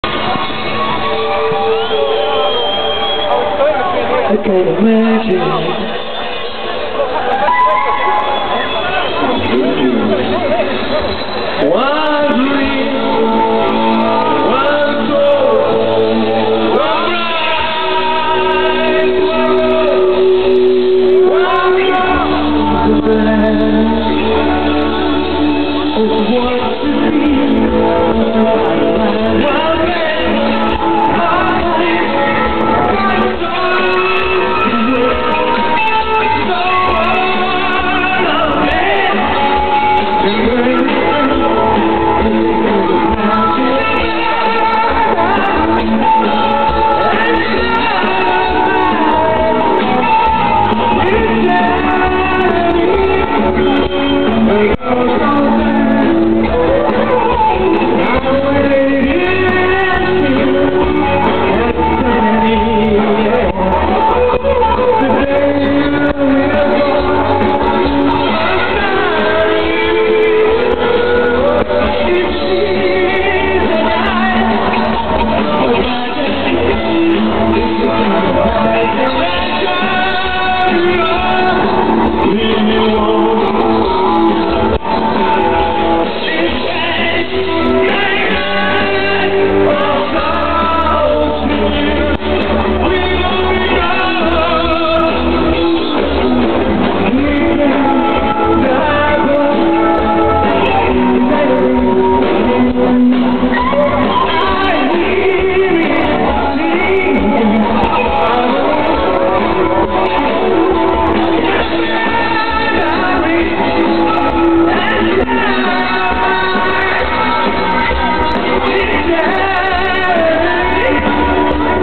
Liverpool Pier Head/Queen tribute Band
This was taken at the Pier Head stage in Liverpool 2004, during the famous Matthew Street Music Festival weekend. A tribute band trying and I say that loosly ...trying to do a Queen number, sound not too good but scenery good and gives you a feel of the excitement of the occasion.